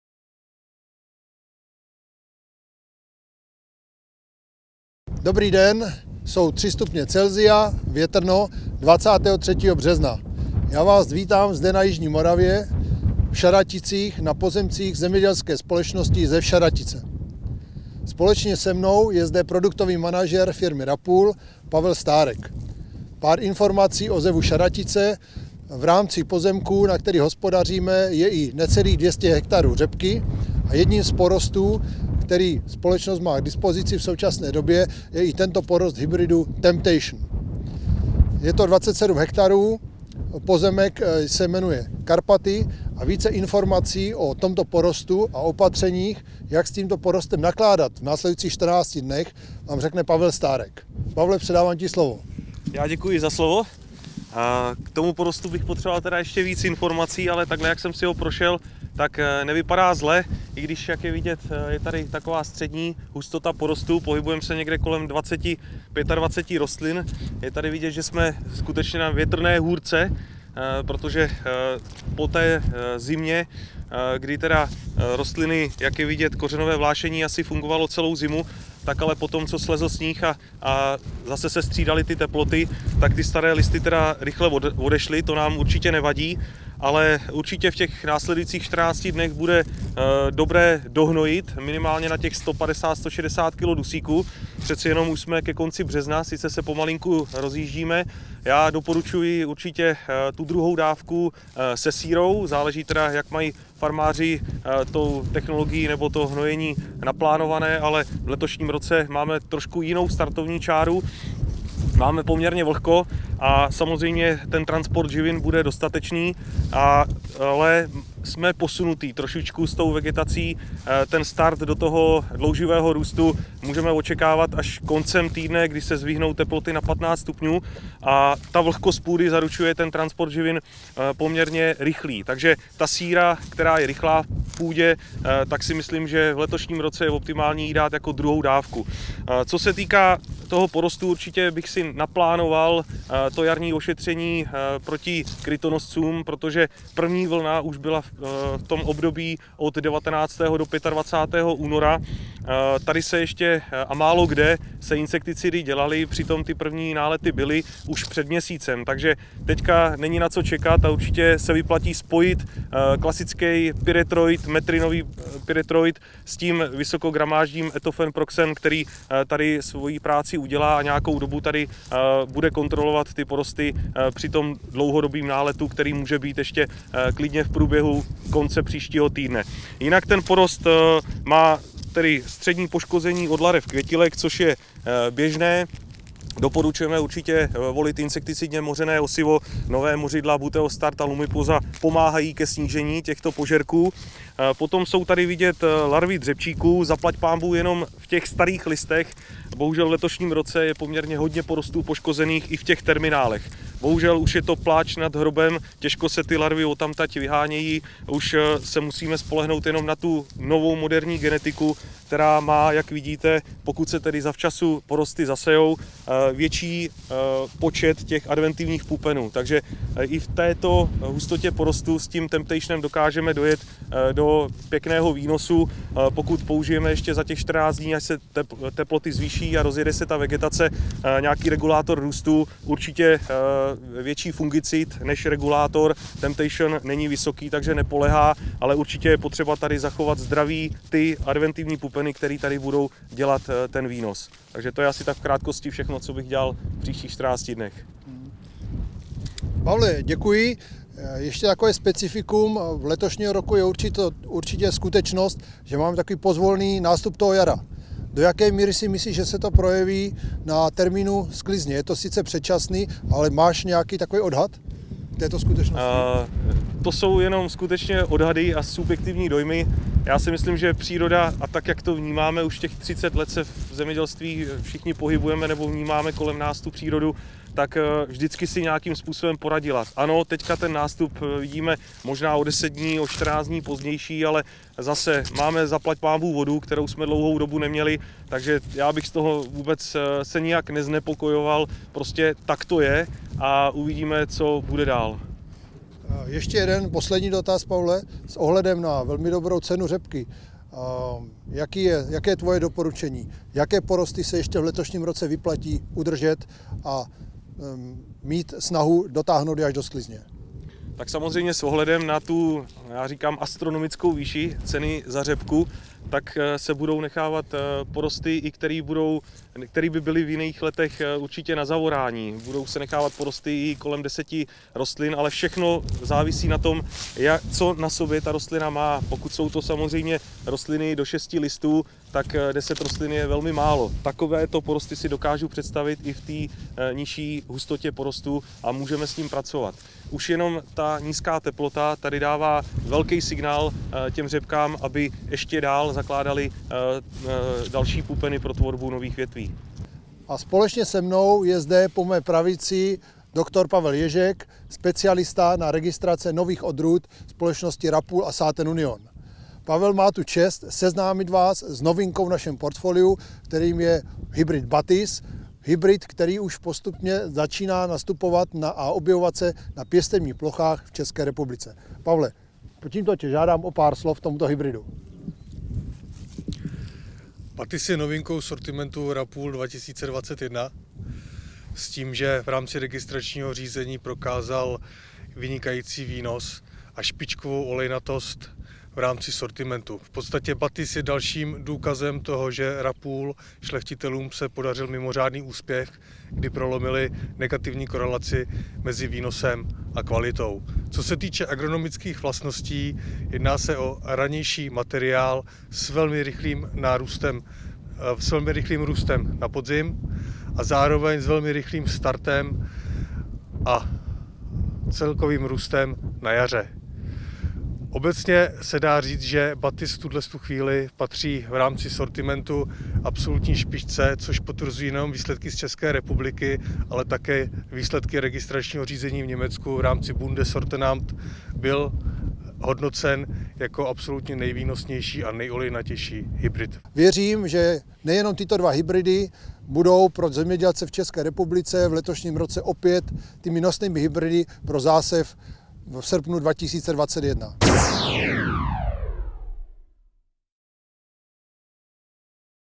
Tradiční jarní inventarizace porostů řepky ozimé 2021
Produktový manažer firmy RAPOOL  Bc. Pavel Stárek vás provede jarní inventarizací porostu řepky ozimé, hybridu TEMPTATION na pozemcích společnosti ZEV Šaratice.
Vzhledem k chladnému počasí, nízké teplotě půdy a velmi pozvolnému nástupu jarní vegetace, je růst a vývoj porostů zpomalen. Z toho důvodu bude vhodná stimulace rostlin dohnojením dusíkem, sírou a dalšími prvky. Seznámí vás také se zvládnutím ochrany proti škůdcům, zejména stonkovým krytonoscům a doporučí volit insekticidně mořená osiva pro letošní srpnové zásevy. Nová mořidla Buteostart a Lumiposa vám výrazně pomohou ke snížení požerků způsobených škůdci v porostech.
Ve druhé polovině vás Ing. Pavel Ježek, Ph.D. seznámí s vysoce výkonnou novinkou, moderním hybridem BATIS, který je jedinečným co do výnosu semen, tak do výnosu oleje.

Vaše společnost RAPOOL CZ s.r.o.